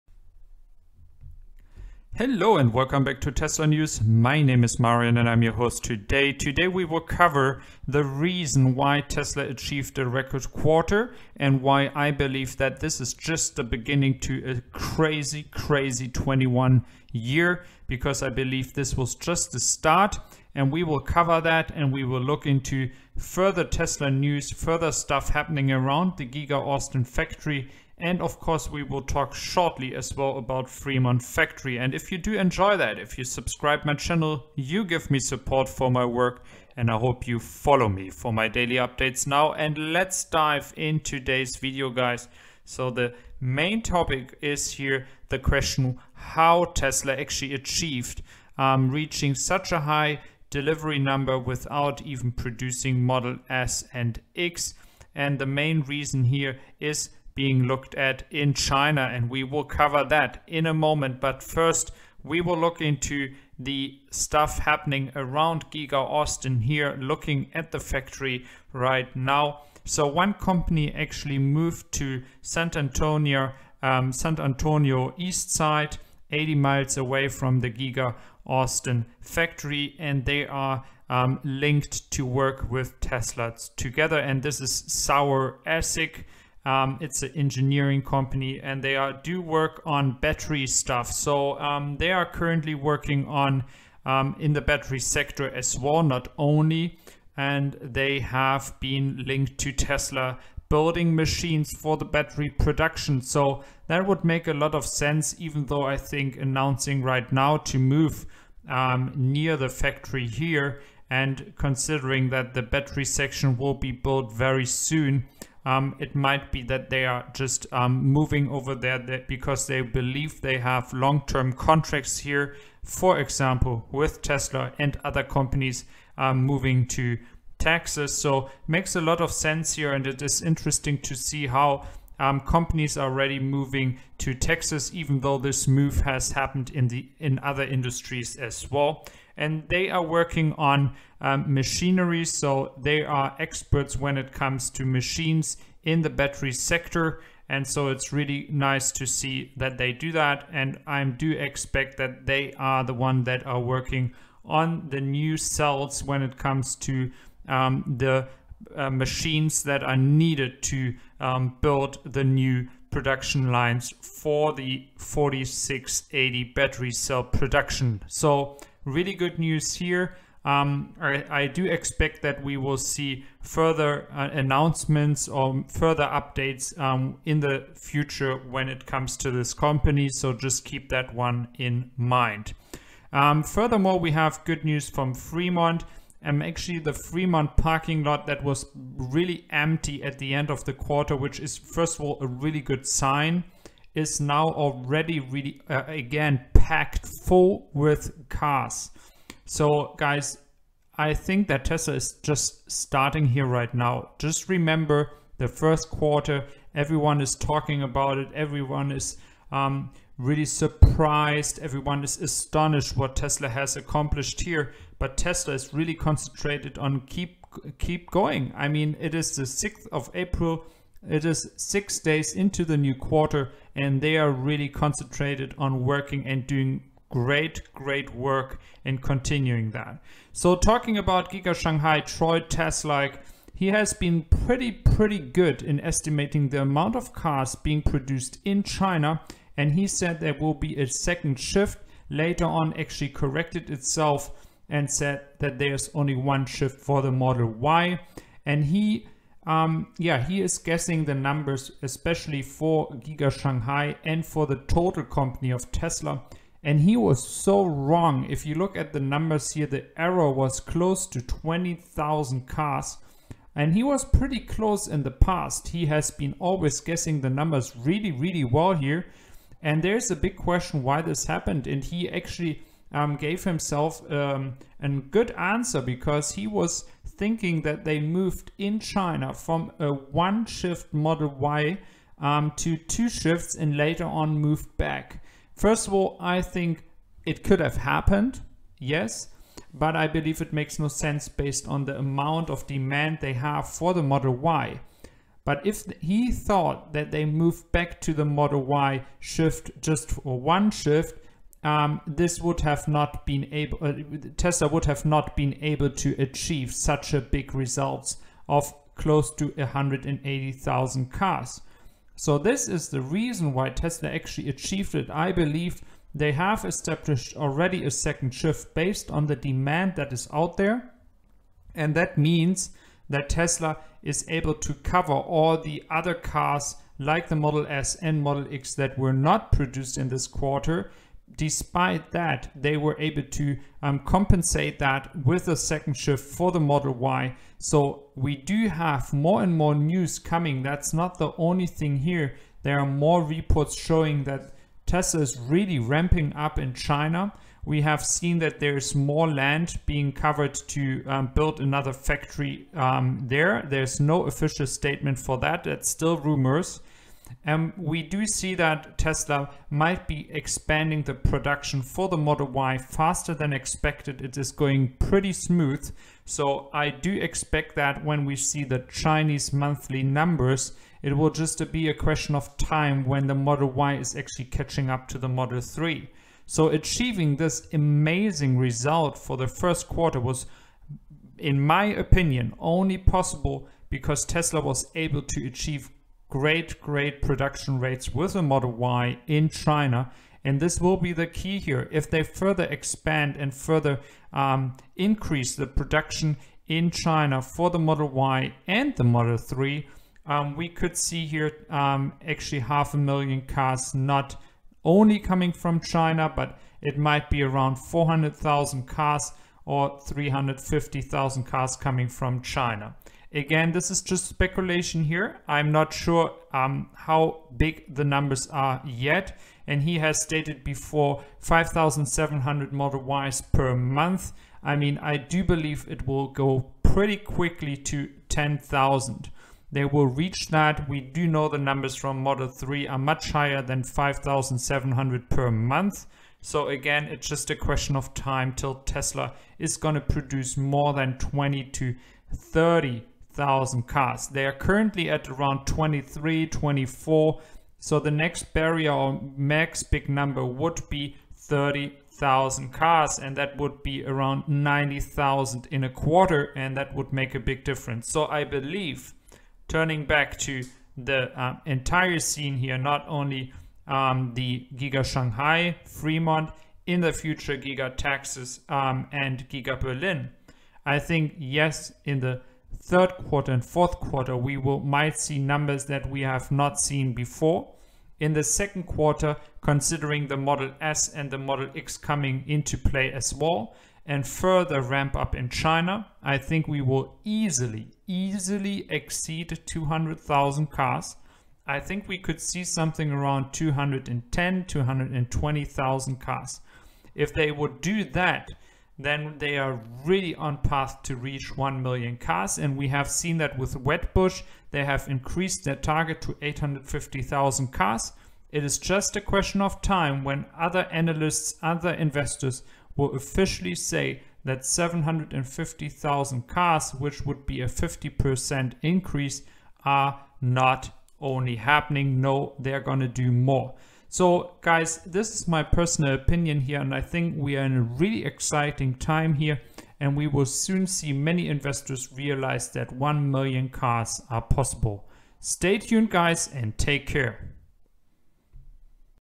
HOW Tesla achieved a RECORD QUARTER, Teslanews, Giga Austin, TSLA STOCK
Hello everyone. Today we cover how Tesla achieved a Record Quarter. Furthermore we will cover other Teslanews, the Giga Austin and TSLA Stock.